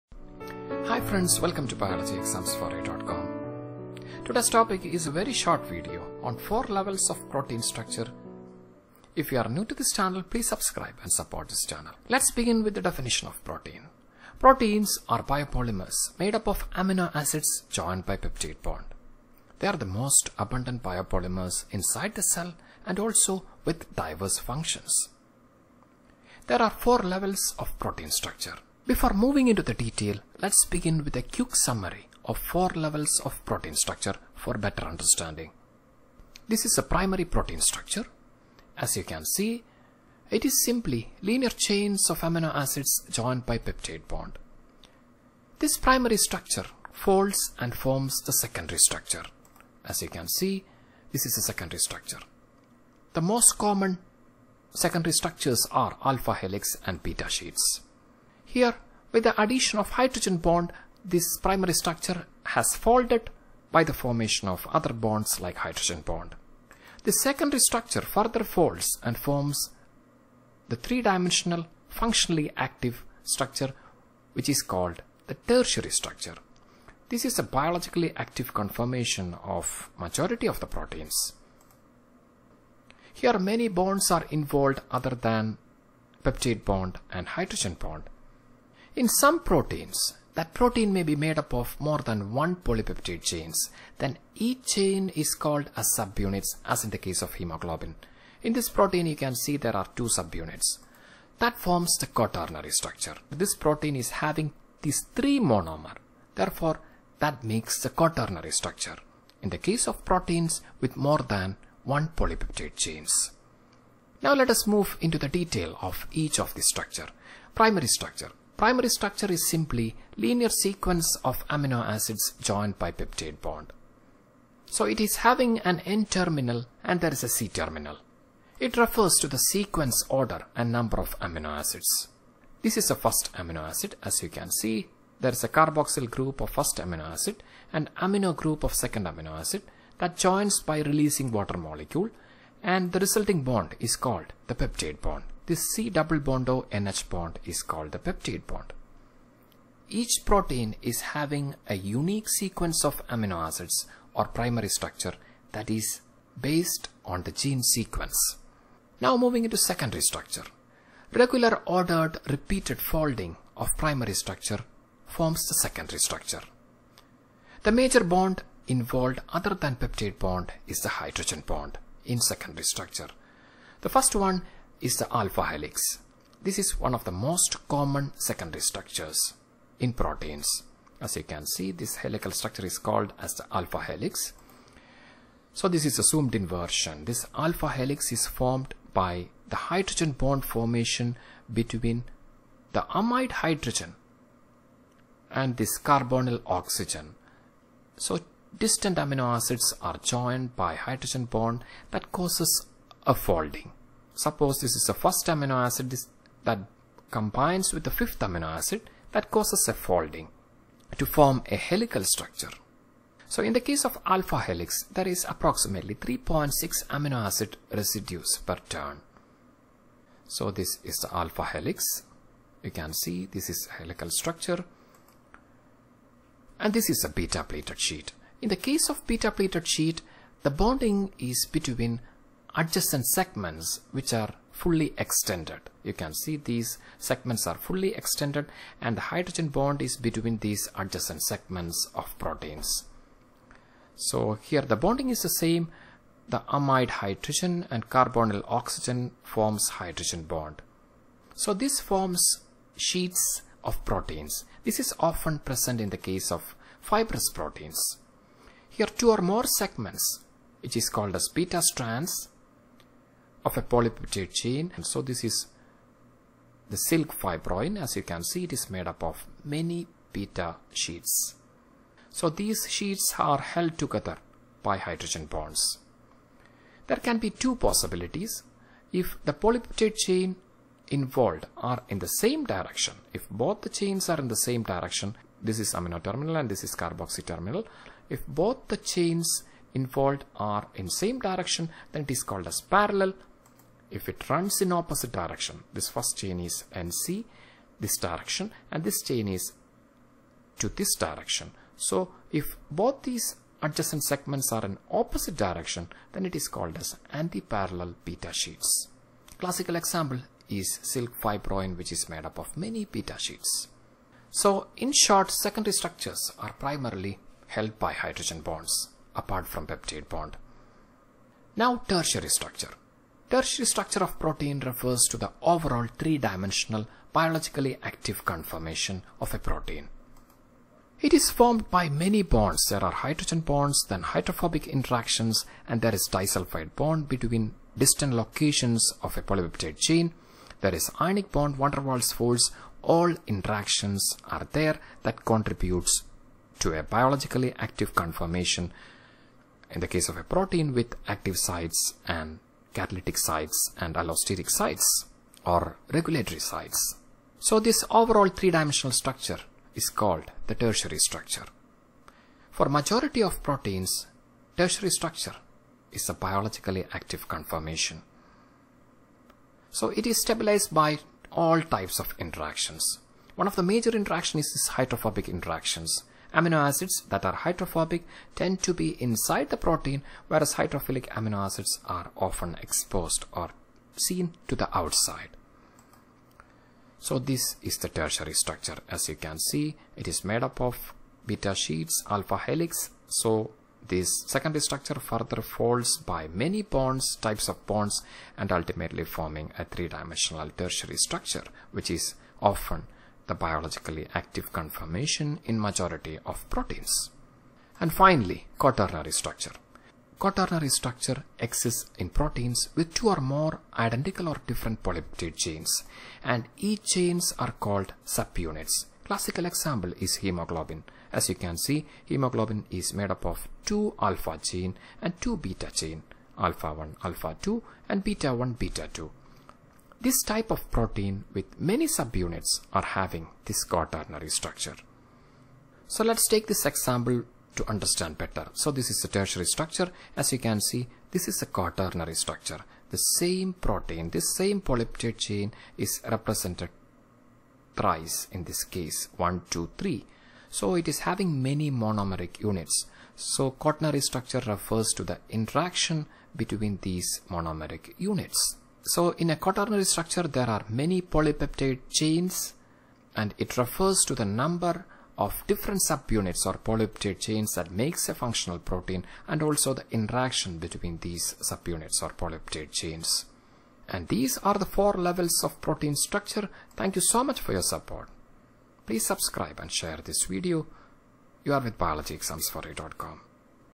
4 Levels of Protein Structure explained with example
8 minute video on 4 levels of protein structure.
Proteins are biopolymers made up of amino acids joined by peptide bond.

Difference between primary secondary tertiary and quaternary protein structures
Click the content below to watch  the relevant sections
0:15 Definition of Protein and Concept summary of 4 levels of protein
1:58 Primary protein Structure, peptide bond and it refers to
2:35 Secondary protein Structure: beta sheets and alpha helix with example
5:20 Tertiary protein structure: 3D structure
6:47 Quaternary protein Structure: with example